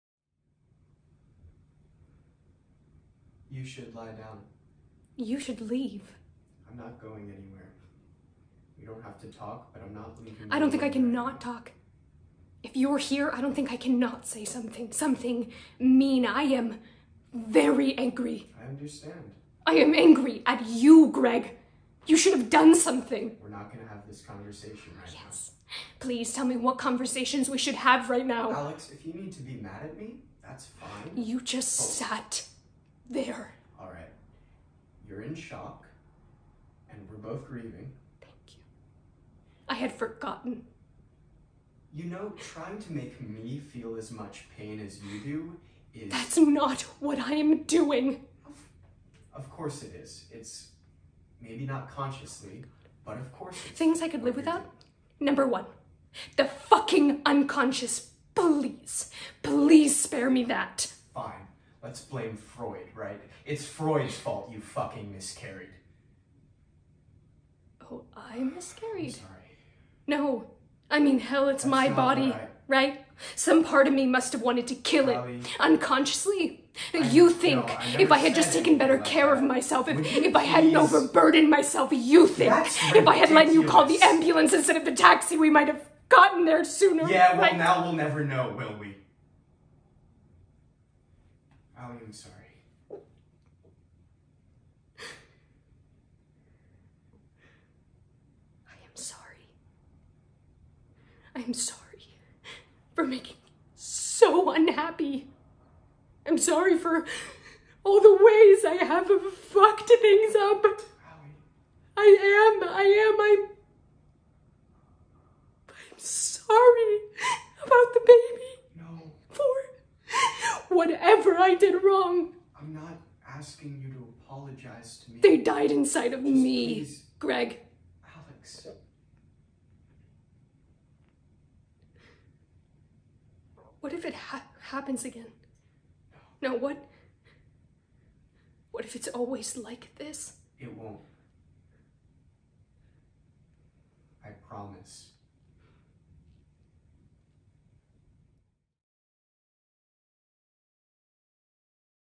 Selected Scene from “A Kid Like Jake” | Magali Trench
A selected scene from “A Kid Like Jake” written by Daniel Pearle, performed by Magali Trench.